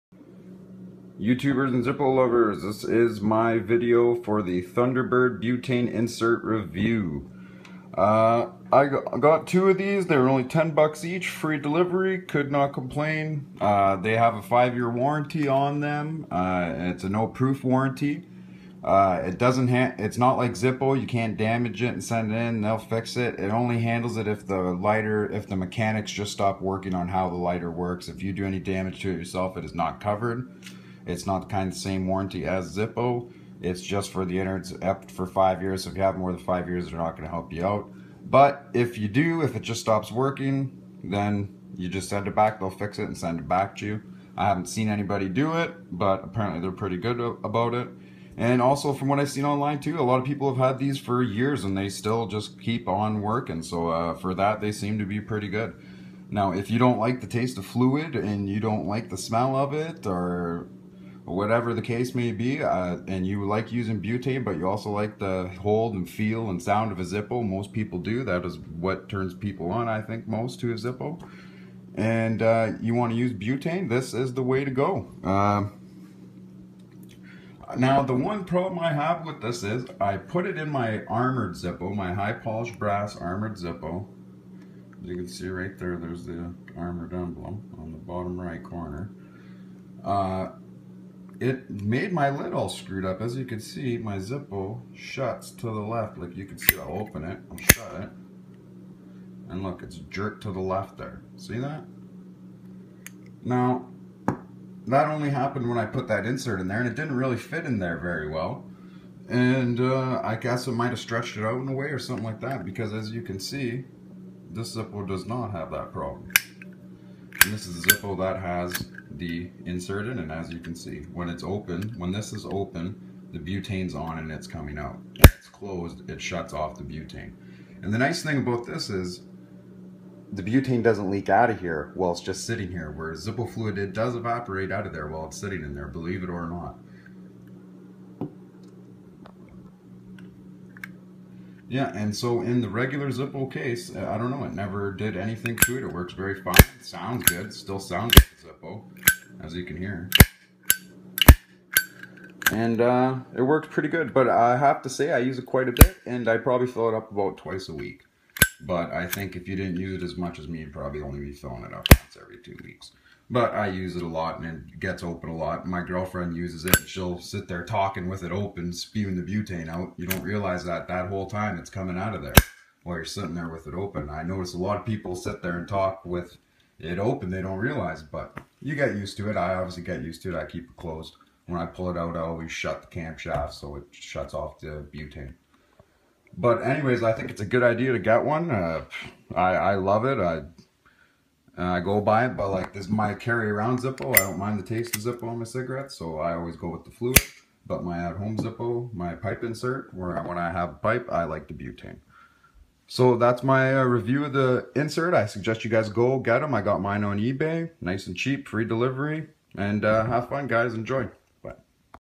Zippo butane insert
Vector thunderbird butane pipe insert. Full review. Must have accessory for pipe smokers